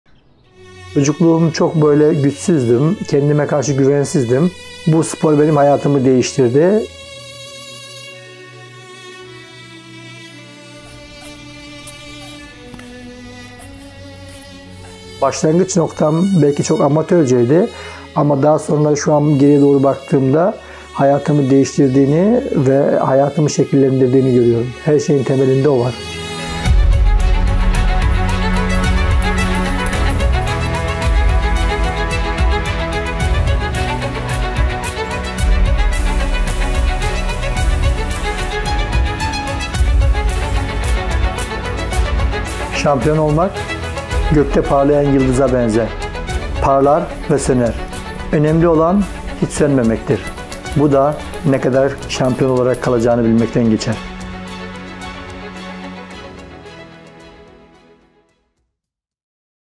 Reebok Spec Ad | Red Komodo
Spec ad for Reebok featuring Cengiz Cumhur

Camera : Red Komodo 6K
Lens  : Irix 45mm cine lens
CREW:
Director : Sebastian Barbaran
DP  : Nicolas Duenas
Gaffer : Alexander Jawfox